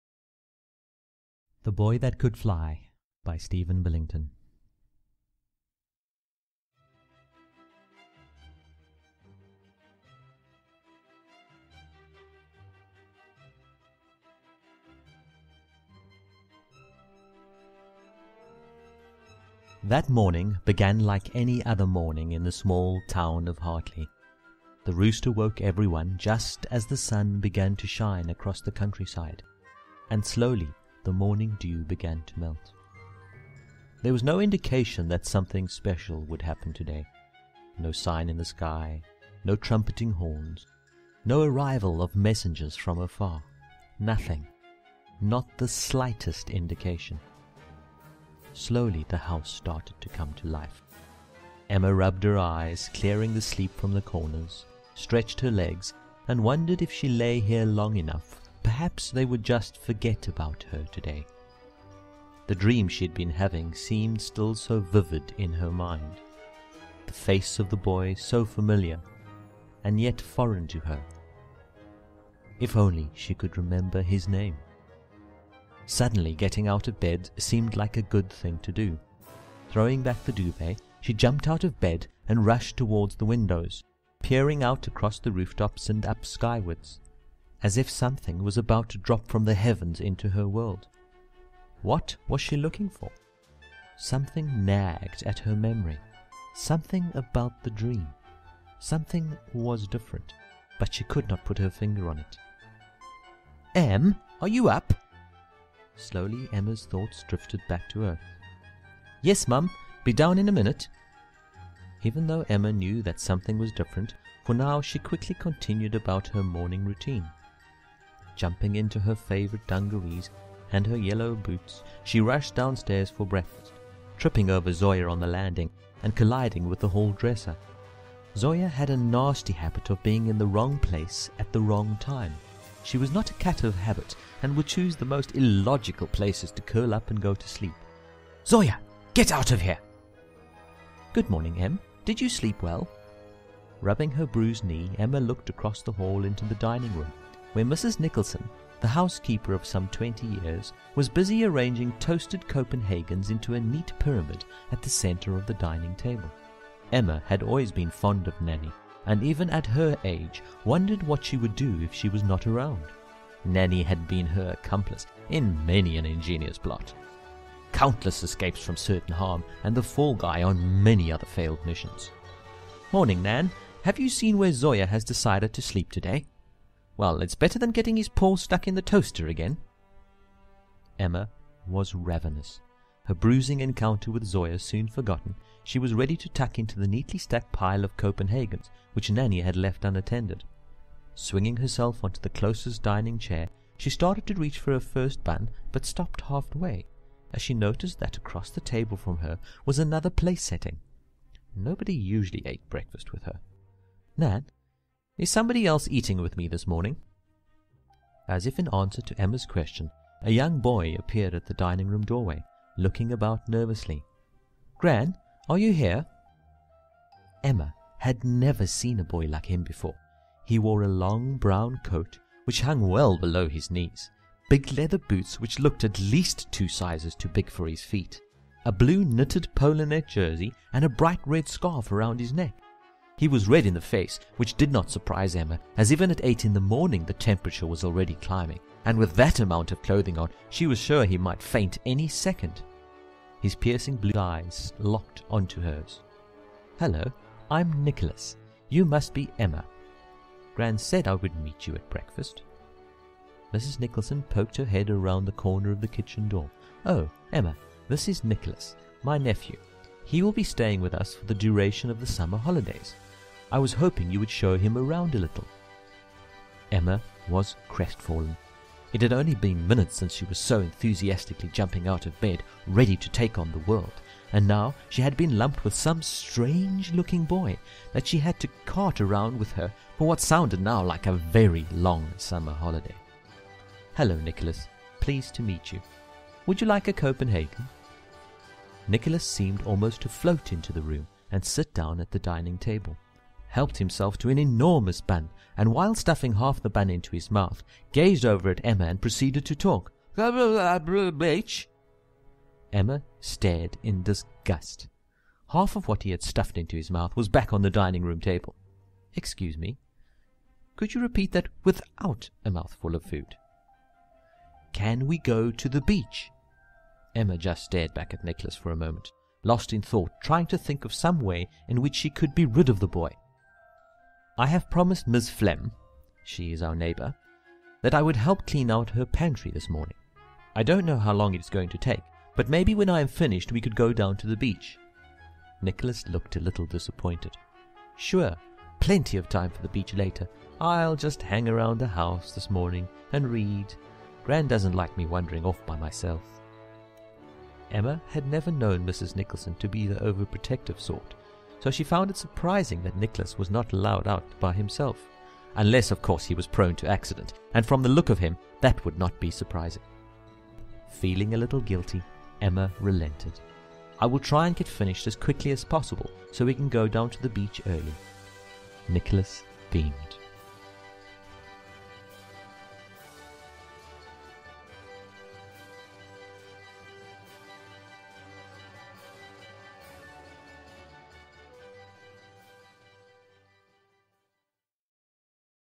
The Boy That Could Fly
Every so often for the last few years I have been writing this story, this year with a little more effort it is nearing completion with a second book in the offing...I wondered what it would sound like so herewith the first chapter. hope you enjoy it.